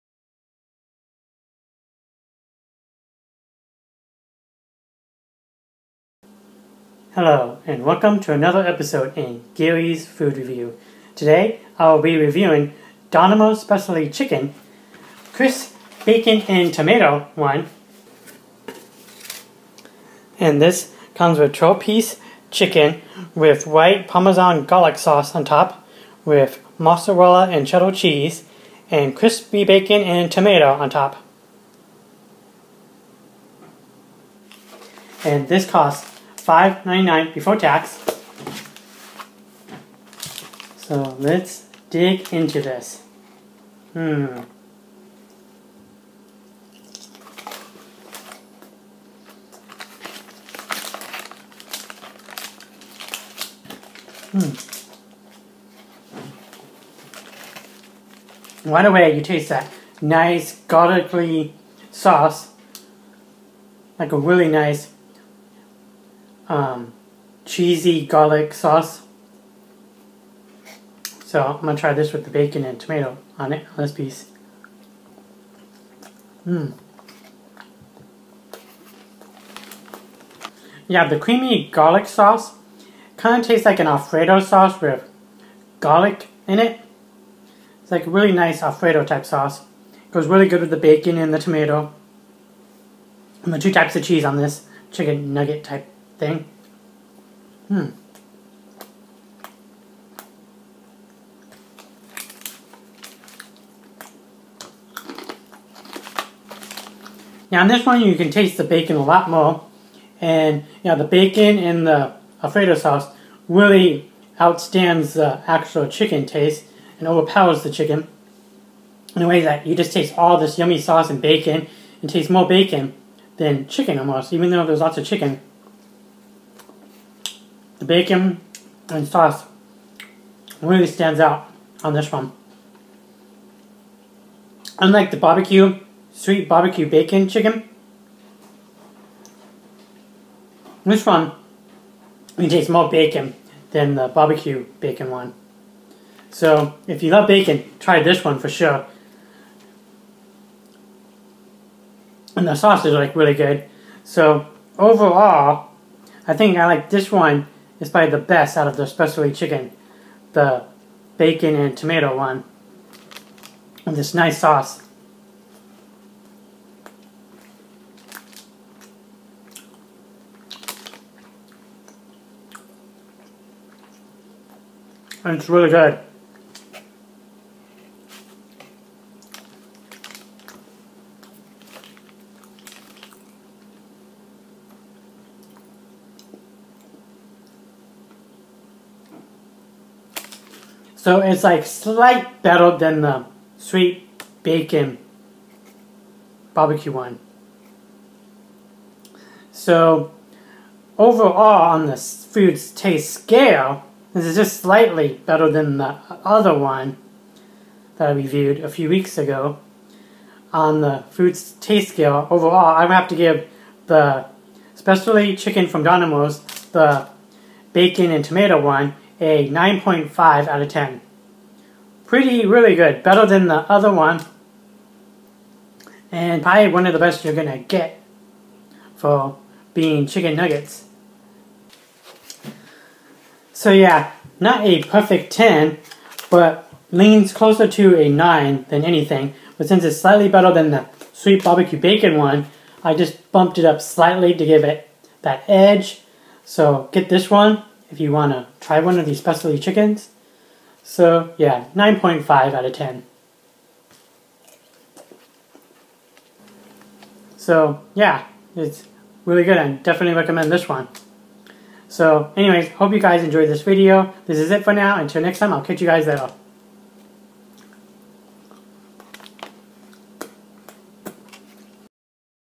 Review: Domino's Specialty Chicken -- Crispy Bacon & Tomato
Tender bites of lightly breaded, 100% whole breast white meat chicken, topped with garlic parmesan white sauce, a blend of cheese made with mozzarella and cheddar, crispy bacon and tomato.